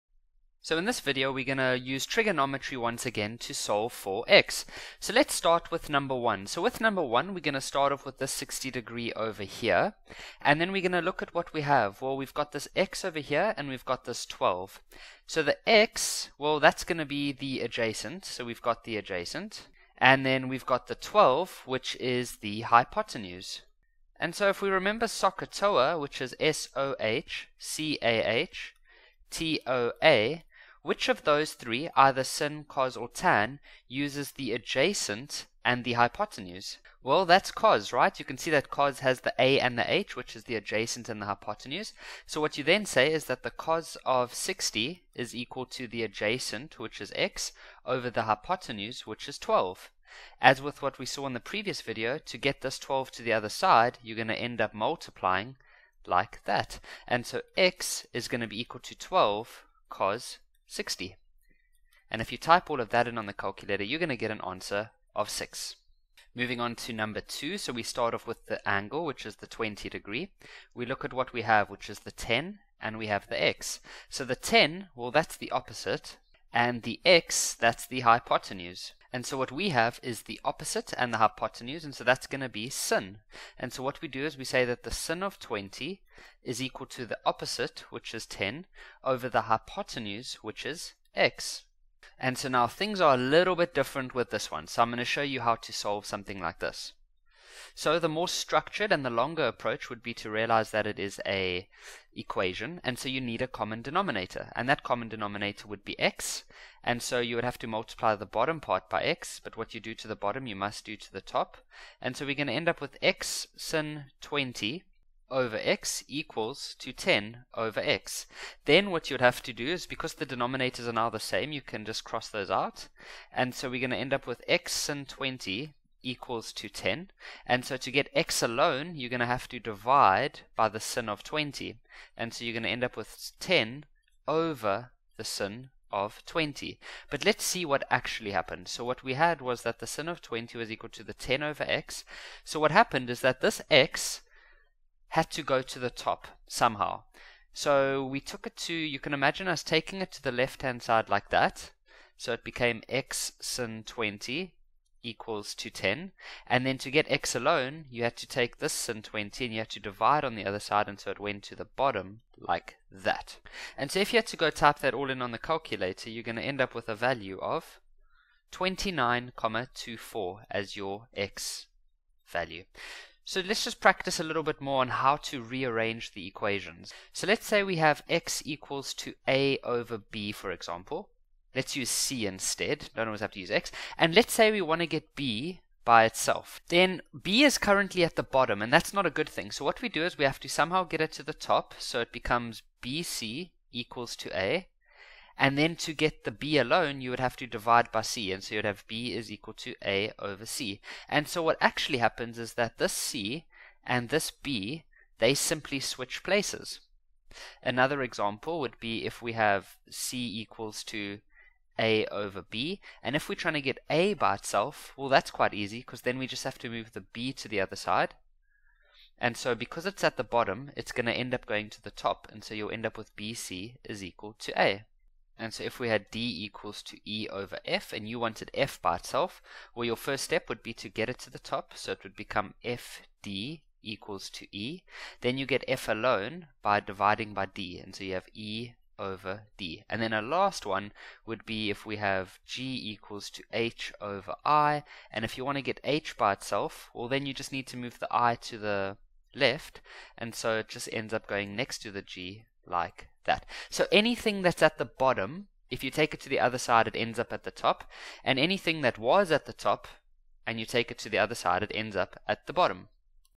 Trig Triangles Grade 10
In this maths lesson we learn how to solve grade 10 trig triangles by using sin, cos and tan.